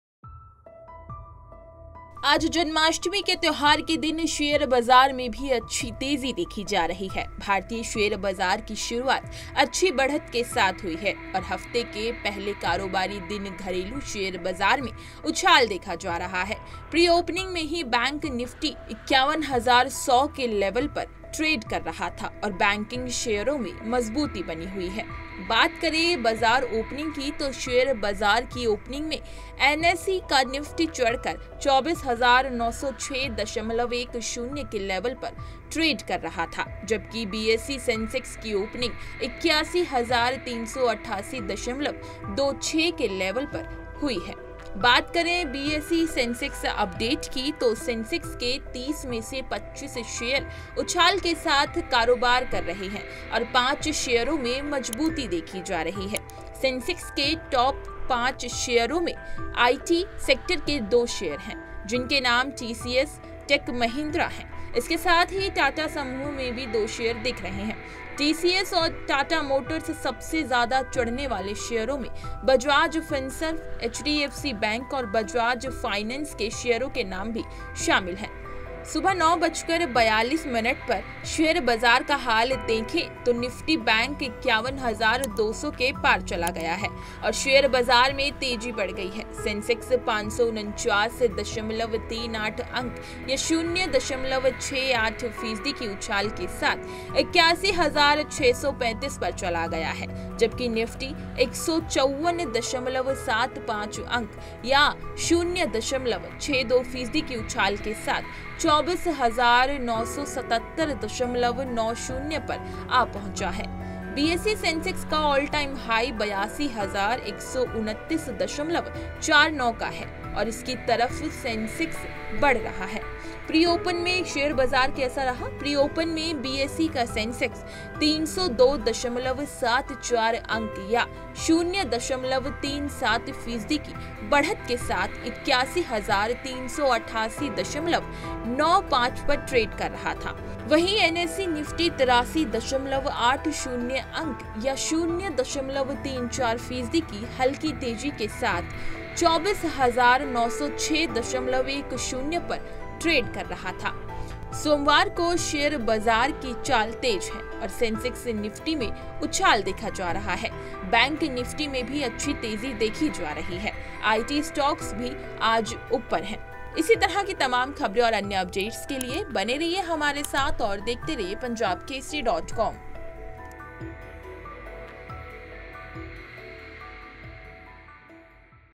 Stock Market News: शेयर बाजार की हुई शुभ शरुआत, तेजी के साथ बाजार में उत्साह | Share Bazaar
सोमवार को शेयर बाजार की चाल तेज है और सेंसेक्स-निफ्टी में उछाल देखा जा रहा है. बैंक निफ्टी में भी अच्छी तेजी देखी जा रही है. आईटी स्टॉक्स भी आज ऊपर हैं.

On Monday, the stock market is moving fast and Sensex-Nifty is seeing a jump. Bank Nifty is also seeing a good rise. IT stocks are also up today.